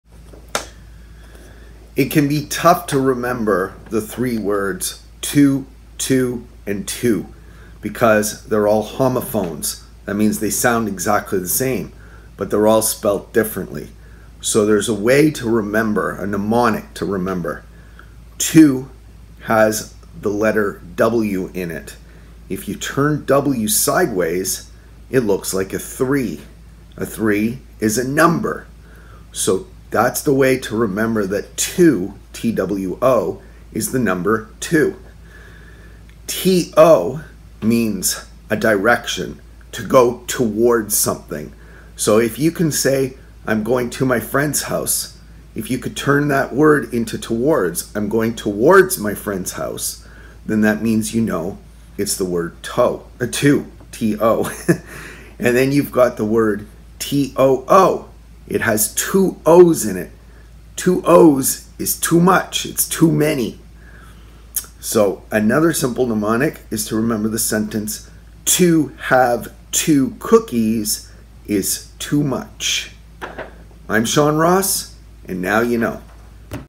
How to remember difference between two, too, and to
Mnemonics to remember the difference between two, to, and two
If you turn the w in two sideways it looks like a 3 which is a number so two is the number 2.
Too has 2 o’s in it which is too much.
To is a direction. If you can remover that to can be interchanged with towards then you will remember to. I went to his house. I went towards his house.